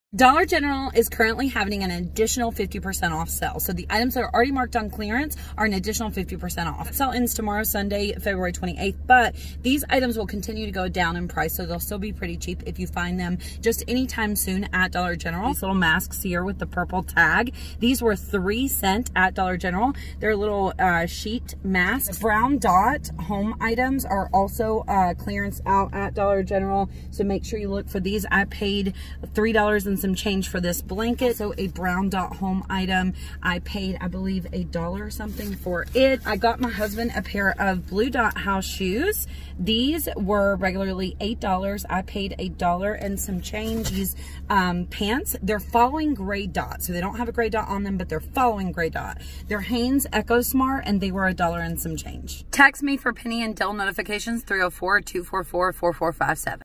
Unmarked Clearance As Low As $0.03 (Dollar General)
Dollar General has tons of clearance right now. I found items as low as $0.03